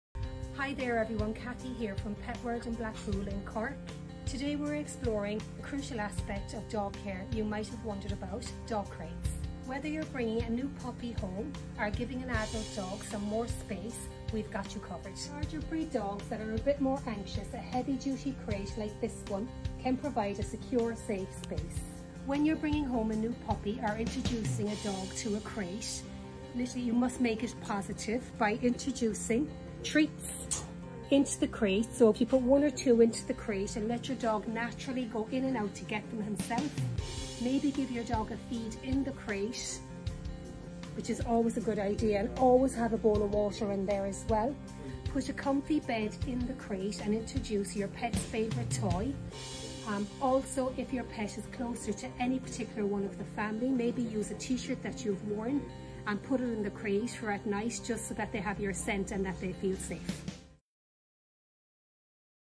Crate Training Tips By Cathy | Petworld
Cathy, our Cork store manager, dives deep into all things crates in our latest video!

From selecting the right size to turning it into a cozy and safe haven for different breeds, she covers it all. Tune in and make your pup's space a comfortable one! 🏠🐶